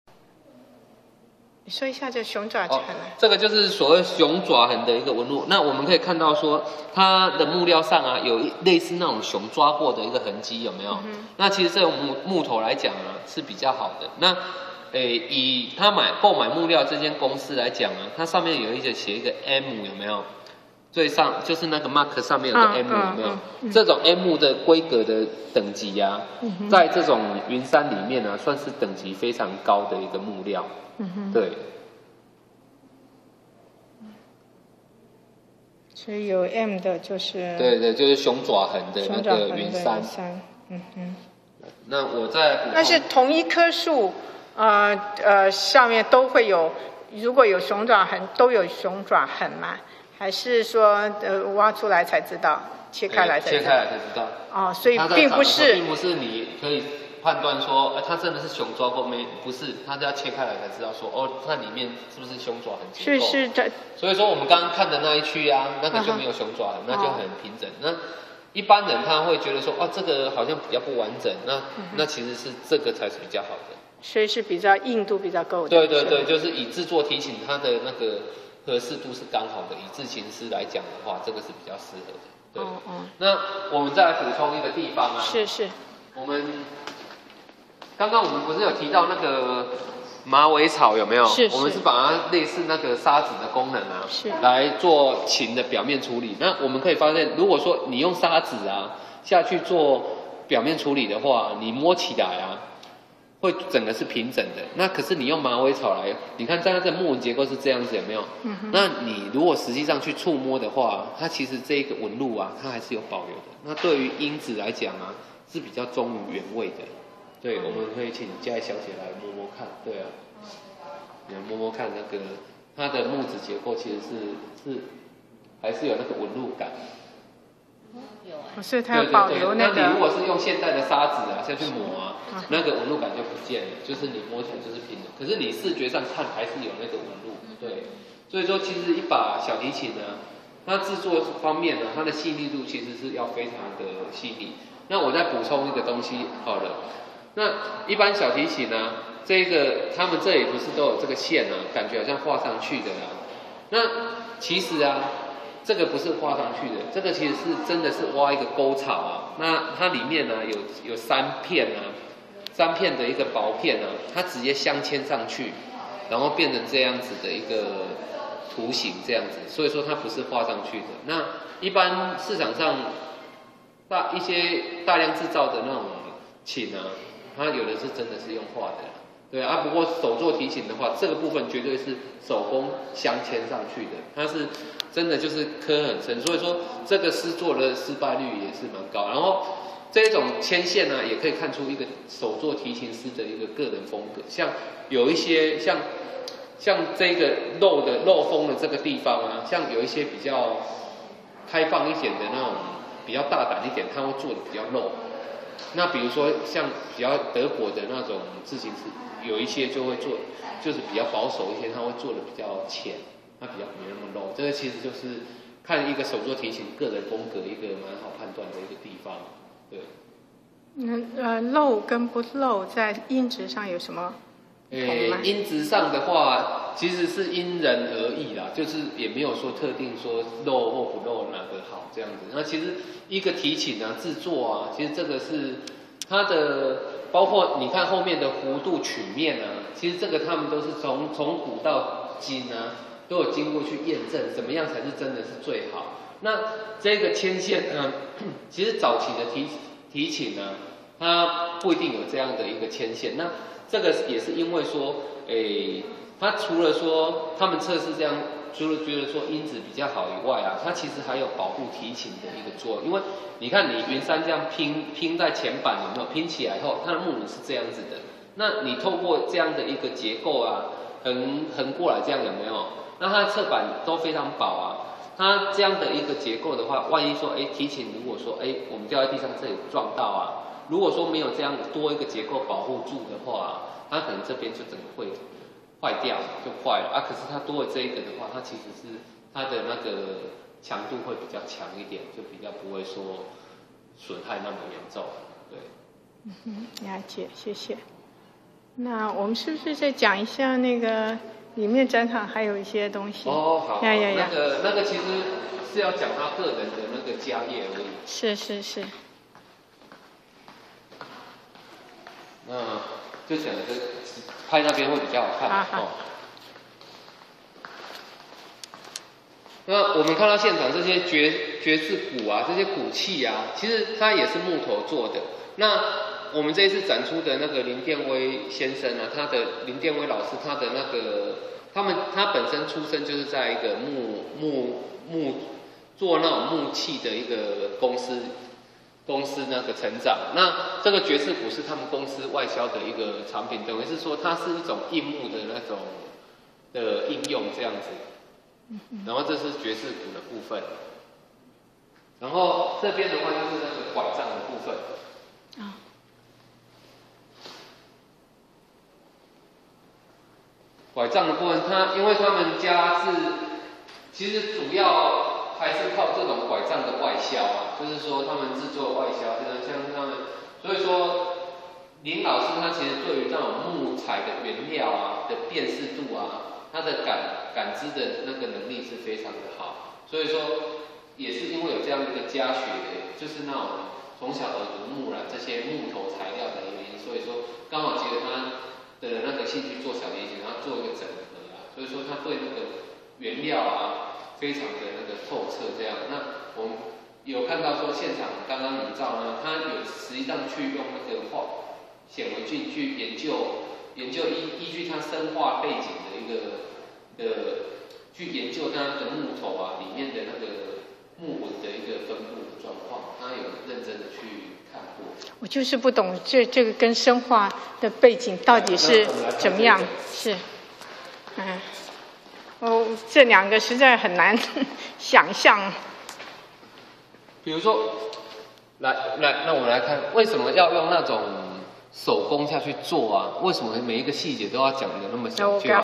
林殿威製琴特色重點介紹VIDEO2429
影音報導『生化碩士林殿威製琴享譽國際興大辦個展』